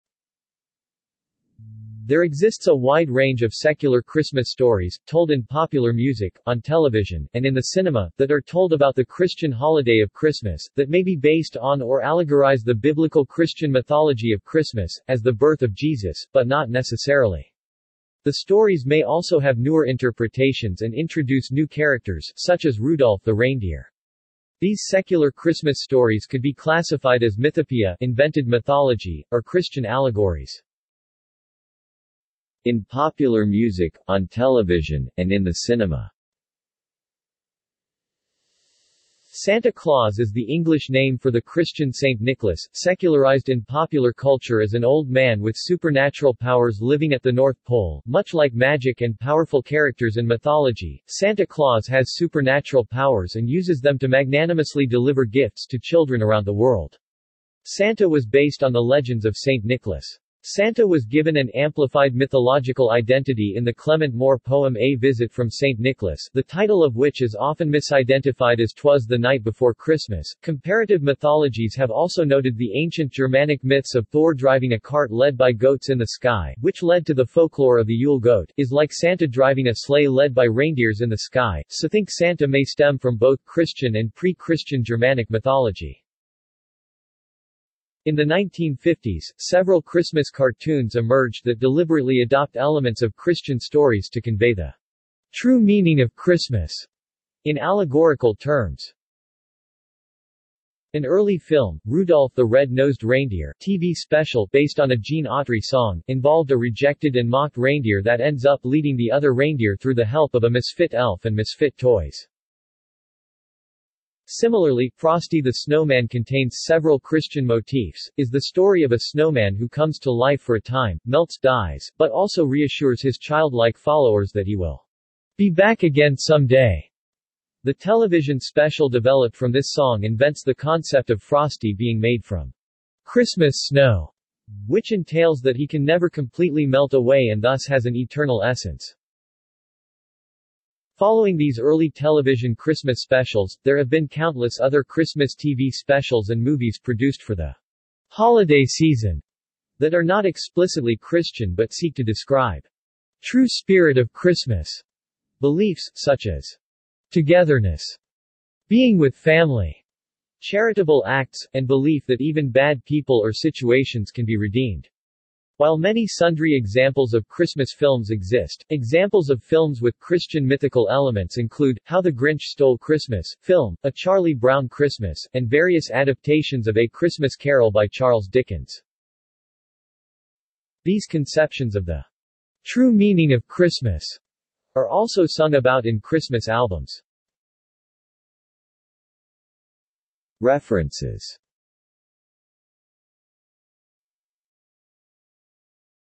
Secular Christmas stories | Wikipedia audio article
This is an audio version of the Wikipedia Article:
Secular Christmas stories

SUMMARY
There exists a wide range of secular Christmas stories, told in popular music, on television, and in the cinema, that are told about the Christian holiday of Christmas, that may be based on or allegorize the biblical Christian mythology of Christmas, as the birth of Jesus, but not necessarily.  The stories may also have newer interpretations and introduce new characters (such as Rudolph the reindeer). These secular Christmas stories could be classified as mythopoeia (invented mythology), or Christian allegories.